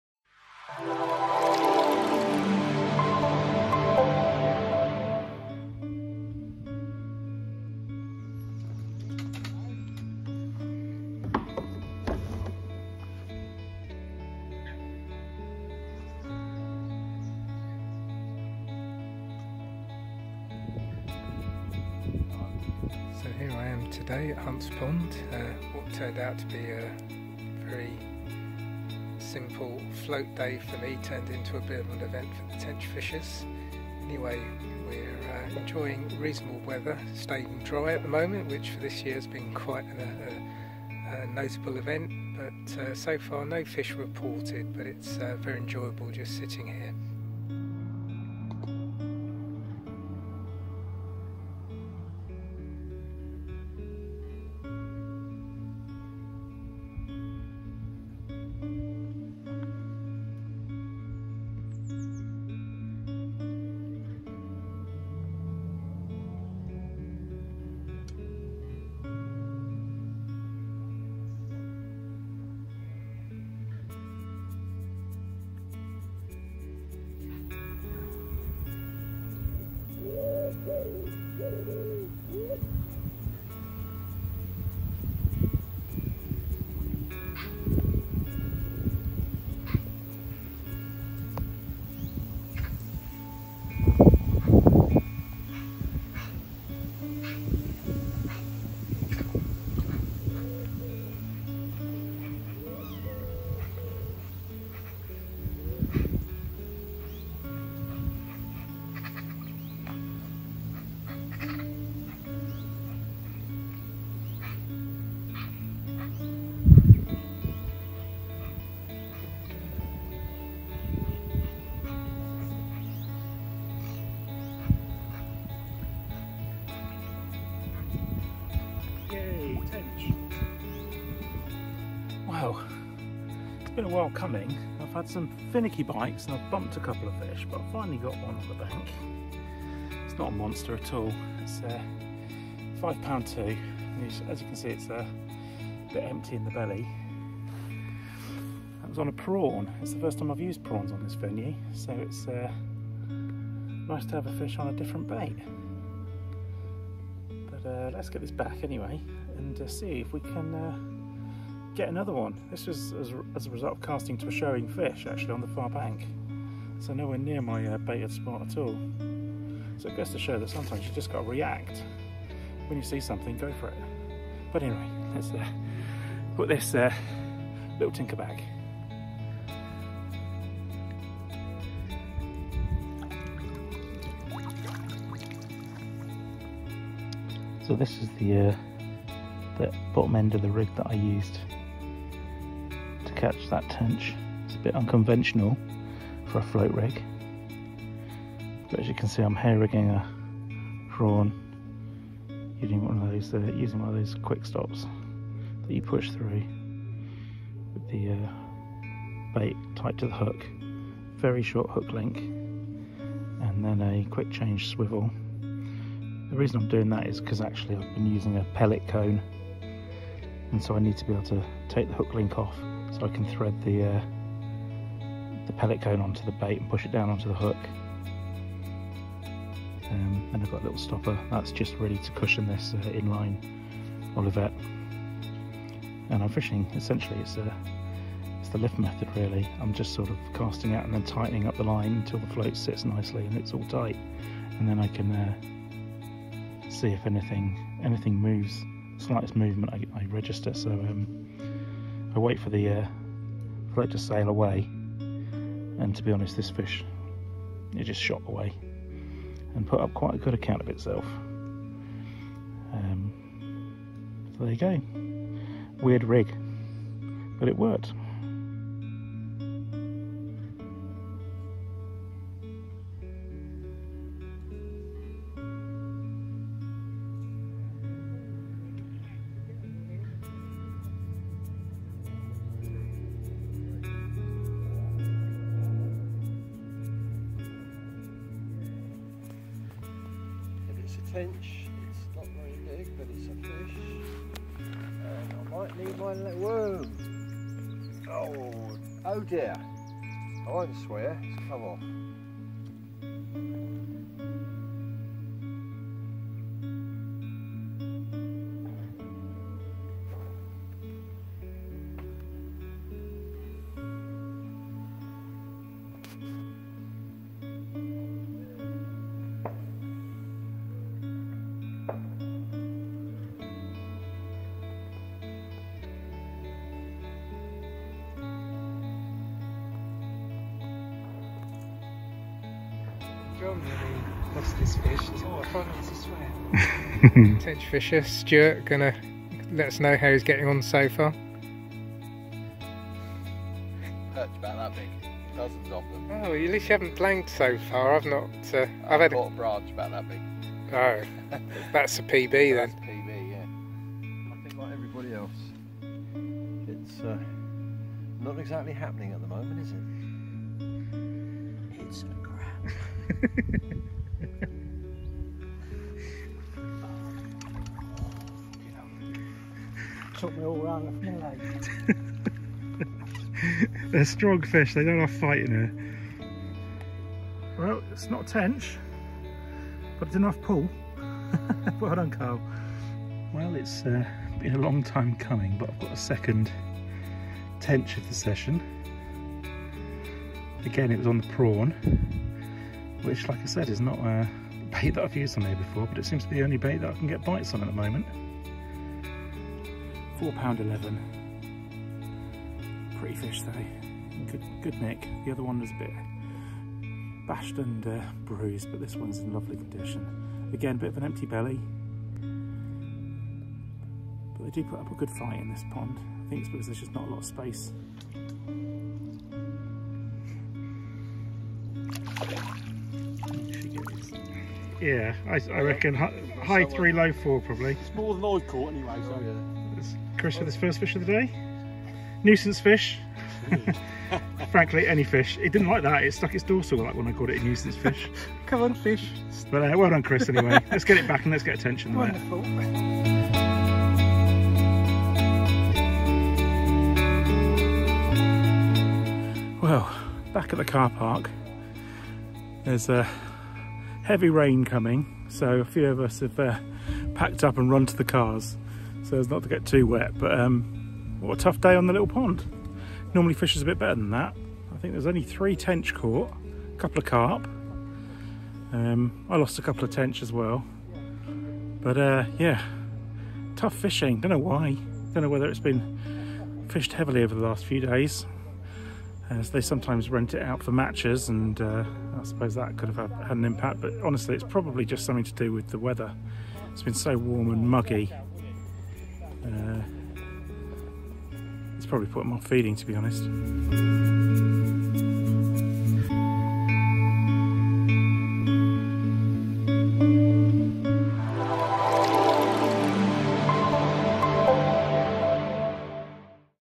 A float fishing only day at Linear Fisheries
A session diary from a Tenchfishers float fishing only event at Linear Fisheries, Nr Witney (Oxfordshire) - August 2023.

For more information about The Tenchfishers, including membership info, Tench rig advice, articles written by members, and details of our other activities please